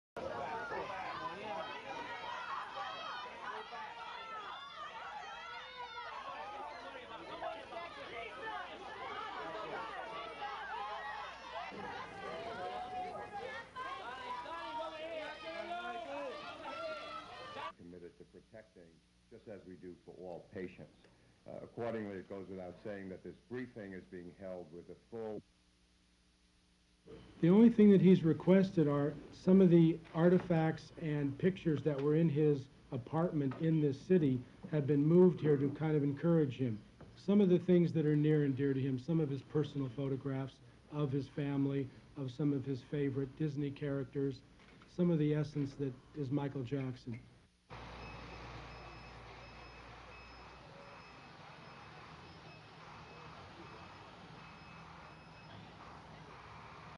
'95 A NEWS CONFERENCE MICHAEL JACKSON'S MEDICAL CONDITION
A NEWS CONFERENCE WAS HELD TODAY, IN WHICH DOCTORS TOLD THE PRESS OF MICHAEL JACKSON'S MEDICAL CONDITION